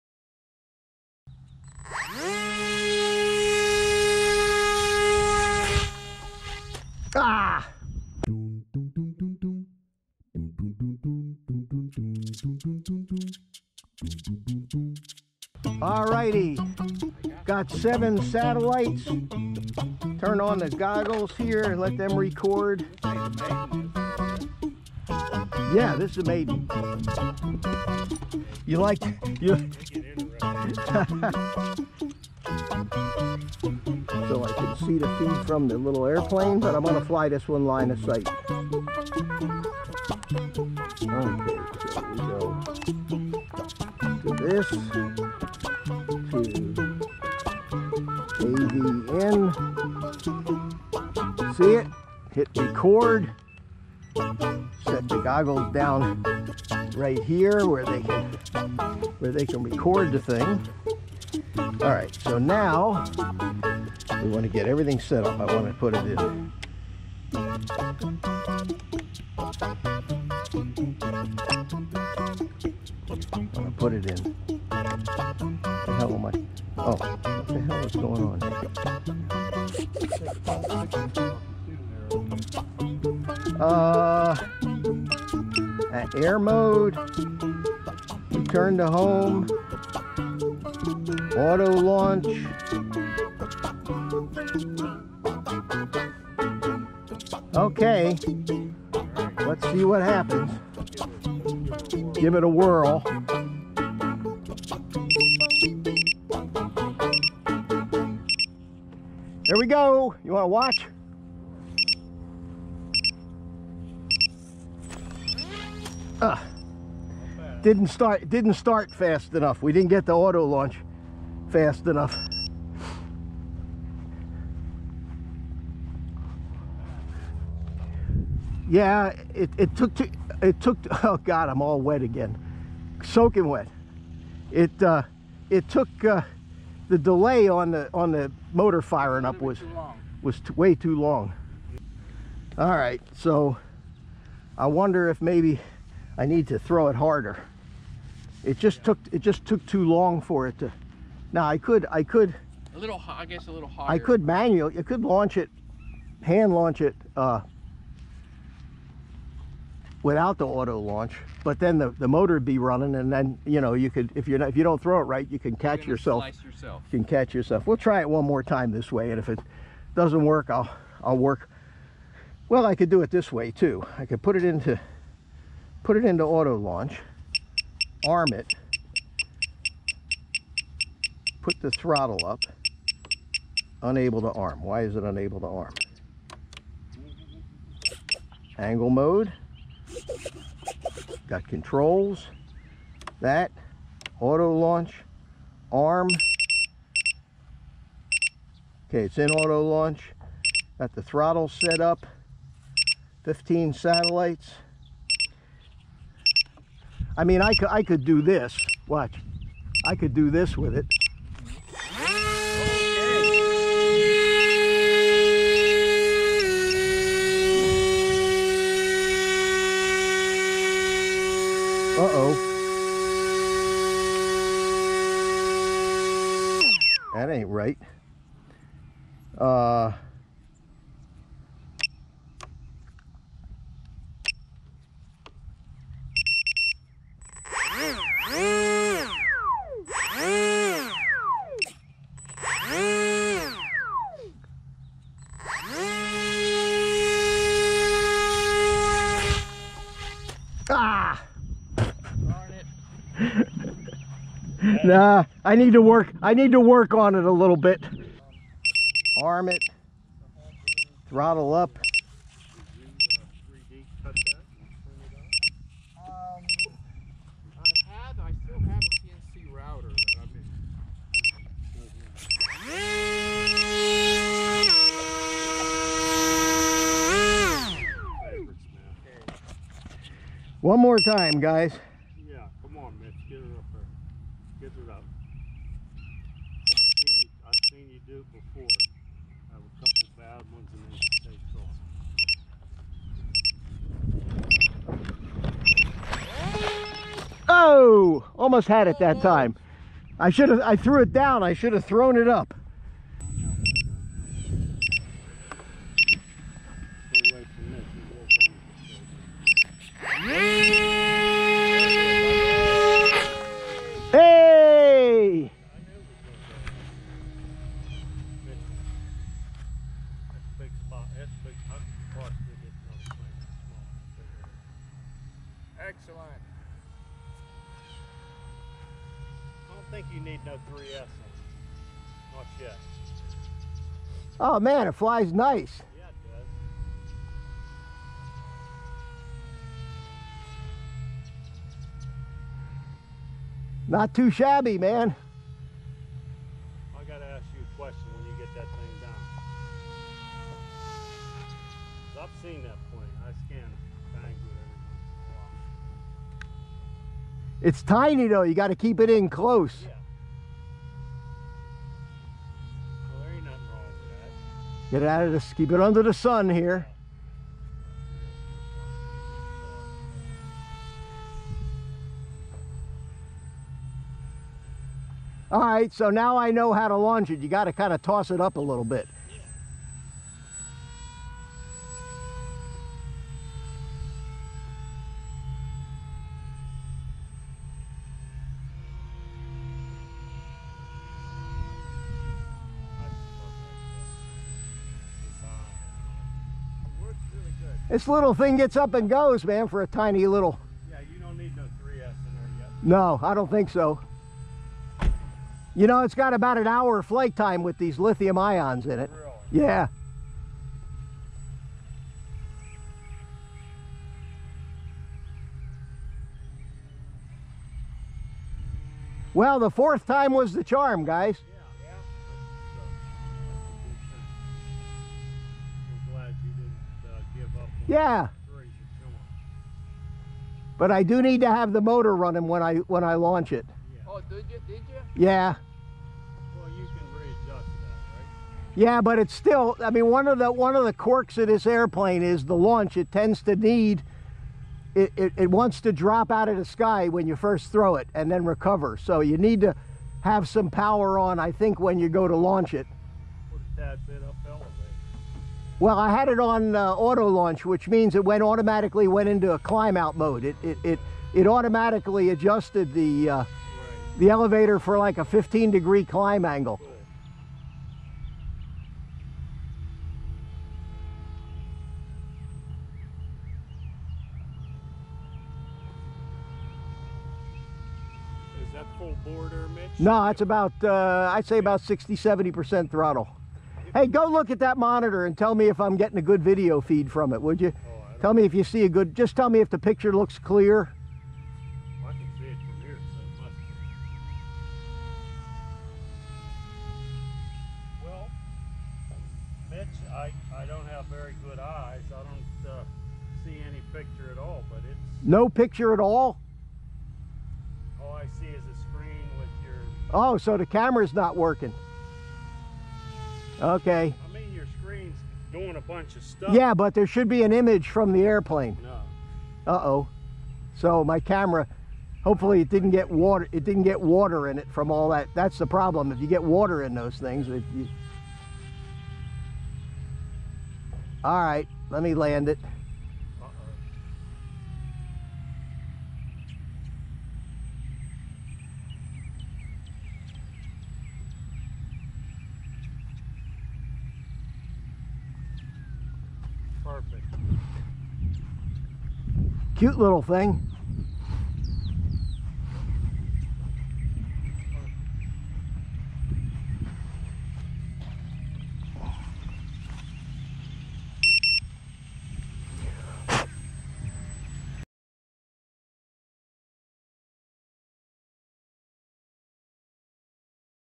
Tiny ZOHD Dart 250 Maiden Flight
Maiden flight of my ZOHD 250g FPV fixed wing. The wing uses a Matek 411 WSE flight controller running INAV 2.5.2. 2 cell 3500mah (18650 lion) pack, Rush Tiny Tank 350mw VTX with Caddx Eos2 nano camera.

Never miss one of my new videos!